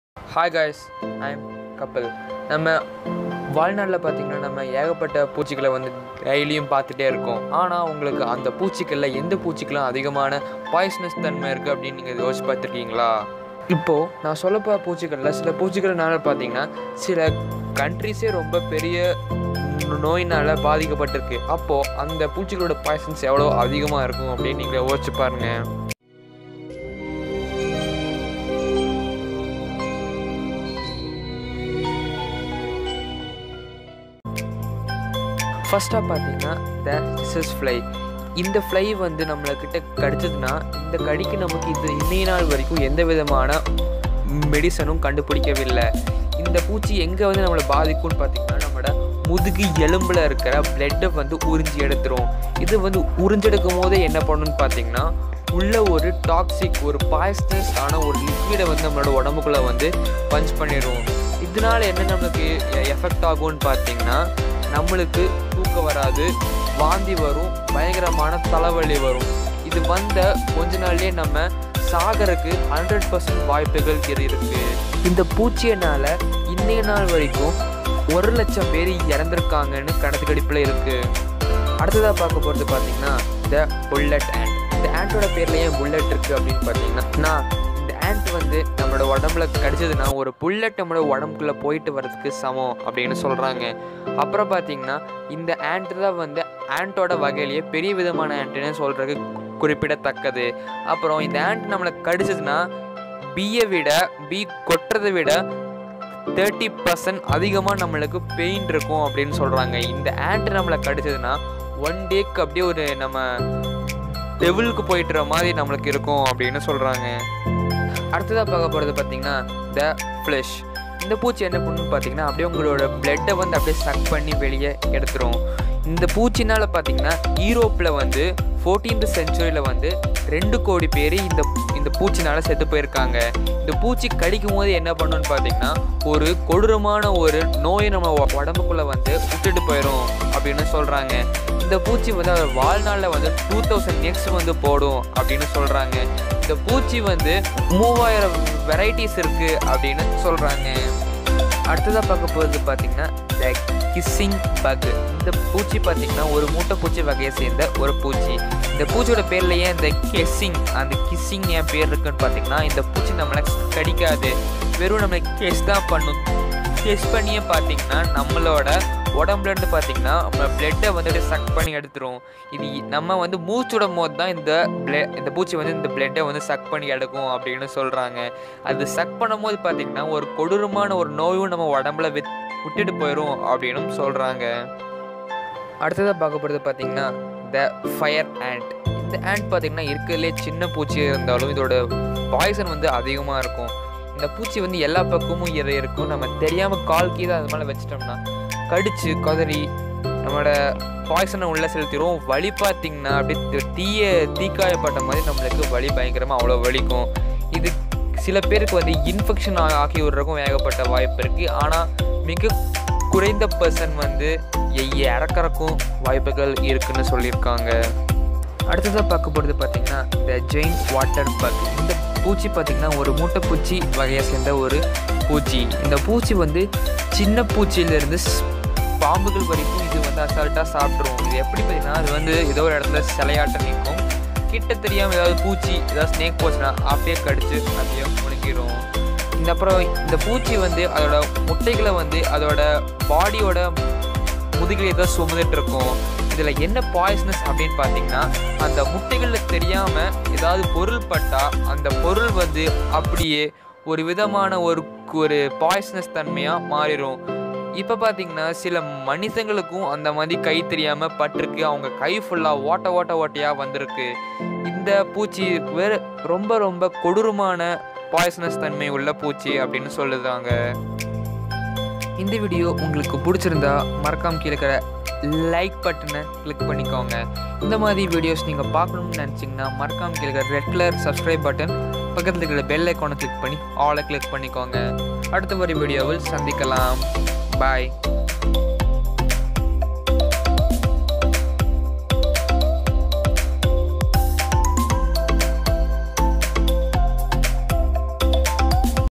உலகின் மிக ஆபத்தான விசப் பூச்சிகள் !! | Most Dangerous Poison Bugs (Insects) !! | Tamil | I'm Kapil
உலகின் மிக ஆபத்தான விசப் பூச்சிகள் !! | Most Dangerous Poisonous Bugs (Insect) !! | Tamil | I'm Kapil

SHORT CONTENT:
                         Hey Guys! This video I shared some most dangerous insect that live along with you.

©I'm Kapil